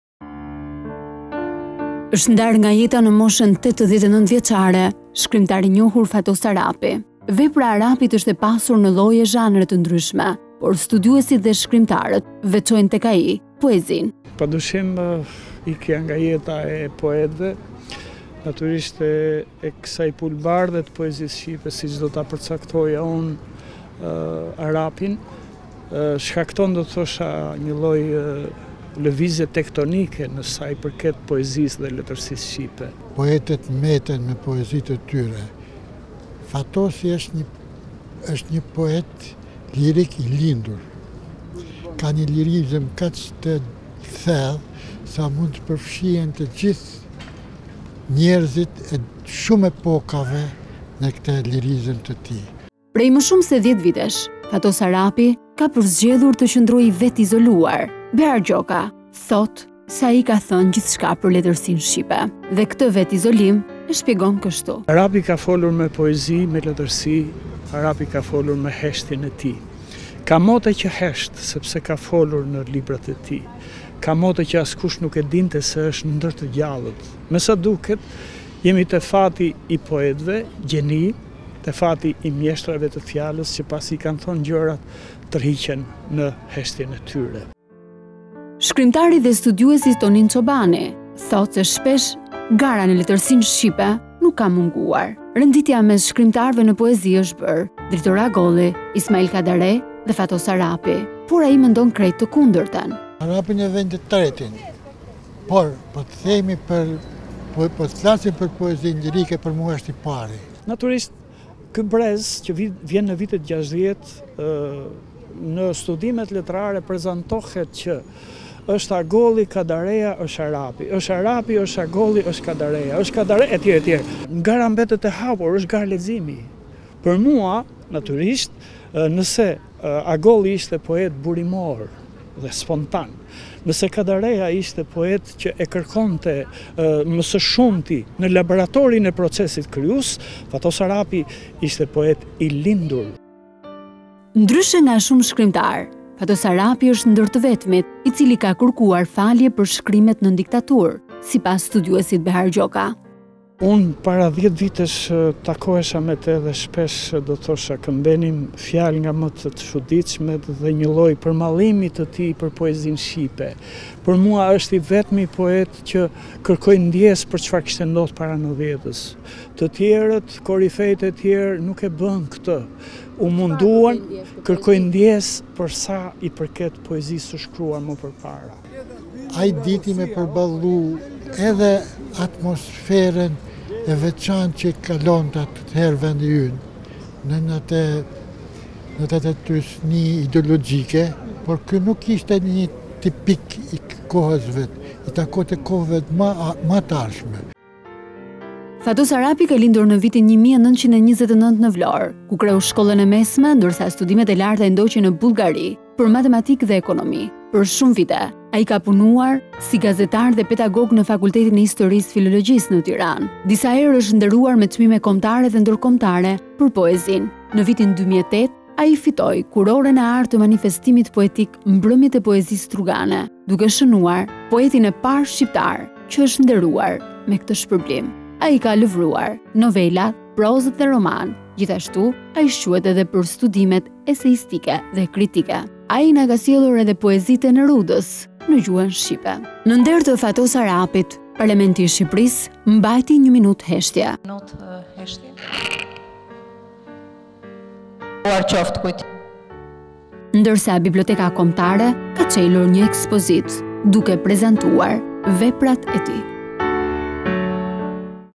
Shuhet ne moshen 89 vjecare shkrimtari Fatos Arapi | ABC News Albania
Shuhet në moshen 89 vjeçare shkrimtari i njohur Fatos Arapi. Studiues dhe shkrimtarë shprehin vlerësime për veprën e tij. Paralamenti mbajti një minutë heshtje, ndërsa Biblioteka Kombëtare, çeli një ekspozitë me veprat e tij.

Është ndarë nga jeta në moshën 89 vjeçare shkrimtari i njohur Fatos Arapi . Vepra Arapit është e pasur në llojet e zhanret e ndryshme, por studiuesit dhe shkrimtarët veçojnë tek ai poezinë.

Behar Gjoka, studiues: “Natyrisht ikja nga jeta e poetëve, e kësaj pulëbardhe të letërsisë shqipe, shkakton një lëvizje tektonikë për sa i përket poezisë dhe letërsisë shqipe.”

Tonin Çobani, shkrimtar: “Poetët mbeten me poezinë e tyre. Fatosi është një poet lirik i lindur. Ka një lirizëm kaq të thellë sa mund të përfshihen njerëzit e shumë epokave në këtë lirizëm të tij.”

Prej më shumë se 10 vitesh Fatos Arapi ka përzgjedhur të qendrojë i vetëizoluar, Behar Gjoka e shpjegon kështu këtë fakt.

Behar Gjoka, studiues: “Arapi ka folur me poezi, me letërsi, arapi ka folur me heshtjen e tij. Ka mote që hesht sepse ka folur me librat e tij. Ka mote që askush se dinte që është ndër të gjallët. Me sa duket jemi tek fati i poetëve gjeni, tek fati i mjeshtrave të fjalës që pasi kanë thënë gjërat tërhiqen në heshtjen e tyre.”

Tonin Çobani thotë se shpesh renditja në letersinë bashkëkohore edhe në poezi është bërë: Ismail Kadare, Dritëro Agolli dhe Fatos Arapi i treti, por ai mendon të kundërtën.

Tonin Çobani, shkrimtar: “Arapin e vejnë të tretin, por po të marrin për nga poezia lirike për mua është i pari.”

Behar Gjoka, studiues: “Natyrisht, ky brezi i viteve ’60, gara ndërmjet shkrimtarëve është e hapur, është garë leximi. Për mua, natyrisht nëse Agolli ishte poet burimor, spontan, nëse Kadareja ishte poet që e kërkonte më së shumti ë laboratorin e procesit krijuae, Fatos Arapi ishte poet i lindur.”

Ndryshe nga shumë shkrimtarë, Fatos Arapi është ndër të vetmit i cili ka kërkuar falje për shkrimet nën diktaturë, sipas Behar Gjokës.

Behar Gjoka, studiues: “Unë para 10 vitesh takohesha me të dhe shpesh herë këmbenim fjalë nga më të çuditshmet he një lloj përmallimi të tij për poezinë shqipe. Për mua është i vetmi poet që kërkoi ndjesë për përpara ’90-ës. Të tjerët nuk e bënë këtë. Ai kërkoi ndjesë për sa i përket poezisë së shkruar më përpara.”

Tonin Çobani, shkrimtar: “Ai dinte të përballonte edhe atmosferën e veçantë që përballonte atëhere vendi ynë, nën atë trysni ideologjike. Por ky nuk ishte një tipik i kohës së vet.”

Fatos Arapi lindi në vitin 1929 në Vlorë, ku kreu shkollën e mesme, ndërsa studimet e larta i ndoqi në Bullgari (Sofje) në fakultetin e Matematikës dhe të Ekonomisë. Ka punuar shumë vjet gazetar dhe pedagog në Fakultetin Histori Filologji të Tiranës. Disa herë ka fituar çmime kombëtare e ndërkombëtare për poezi. Në vitin 2008 e fitoi “Kurorën e artë” në manifestimin poetik “Mbrëmjet e poezisë strugane” dhe është i pari poet shqiptar që e ka fituar këtë shpërblim./abcnews.al